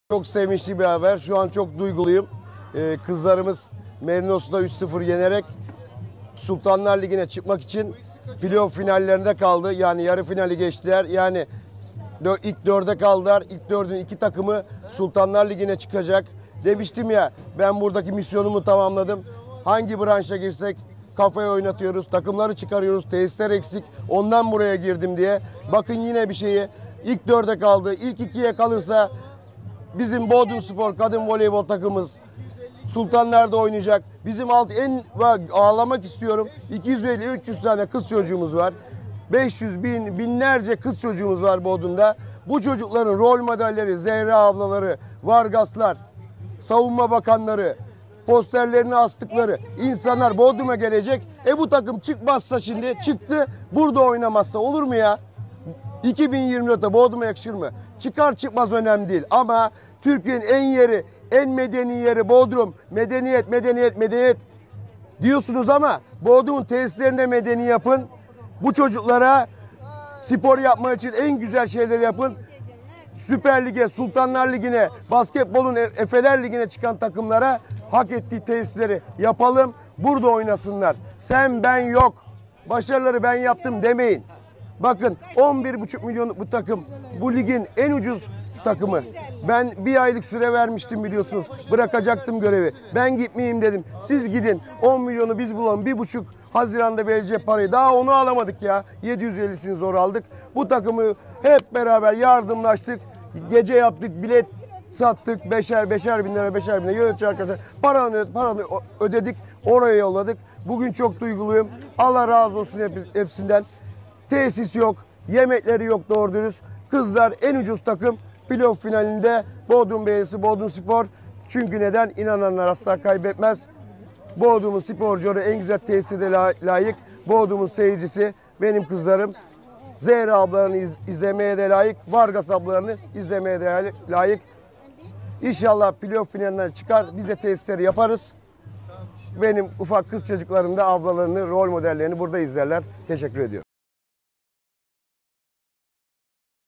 Karakaya; "Bodrum Sporcuları En Güzel Tesislere Layık"
Türkiye Voleybol Federasyonu KFC Kadınlar 1'inci Lig A Grubu'nda mücadele eden Bodrum Belediyesi Bodrumspor bugün Merinos Voleybol maçını 3-0 kazandı. Eski Bodrumspor Başkanı ve Cumhur İttifakı Meclis Üyesi Adayı Rıza Karakaya, kazanılan maç sonrası açıklamalarda bulundu.